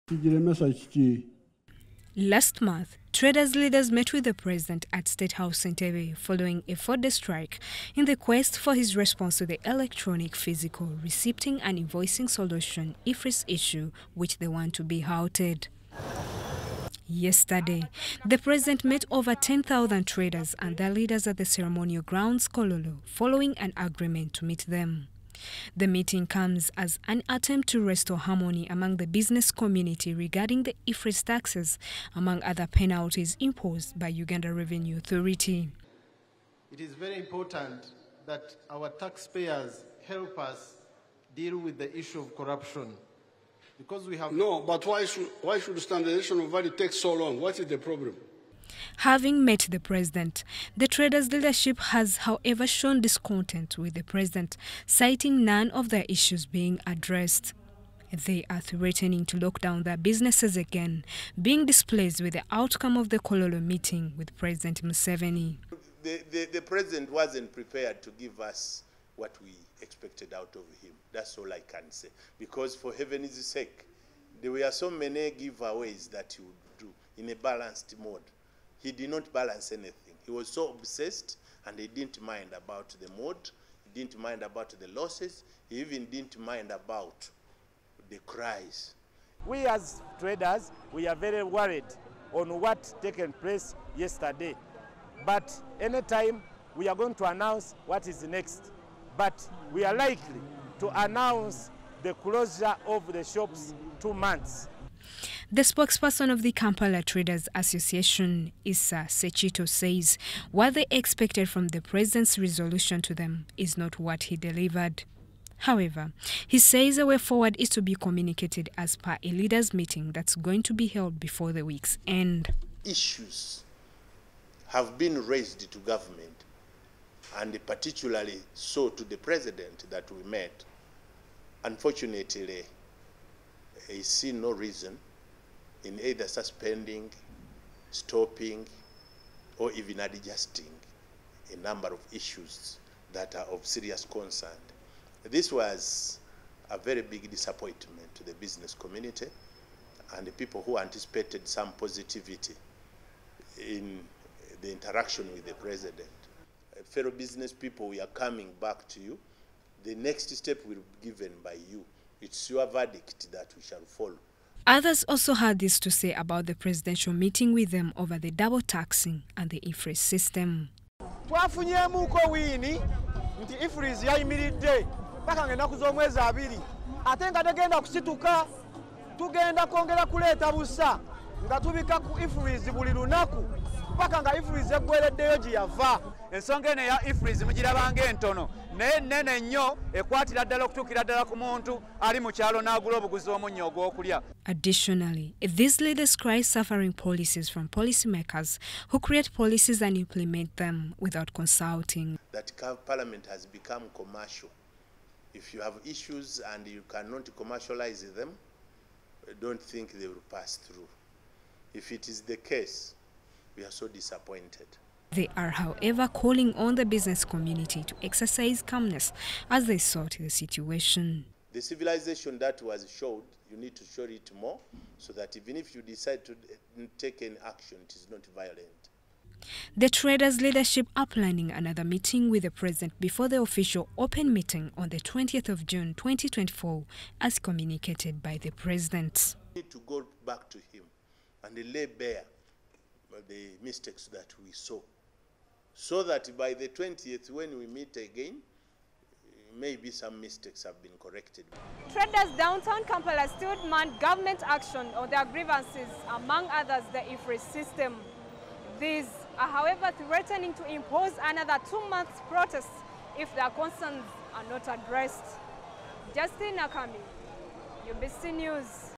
TRADERS THREATEN ANOTHER TWO MONTHS SIT-DOWN PROTEST
Traders in downtown Kampala continue to express dissatisfaction with the resolutions reached during yesterday's meeting with the president, particularly regarding grievances such as the EFRIS system imposed by URA and other penalties.
Leaders from various associations, including KACITA and the Federation for Traders Association, insist on considering another two-month sit-down strike until their concerns are addressed.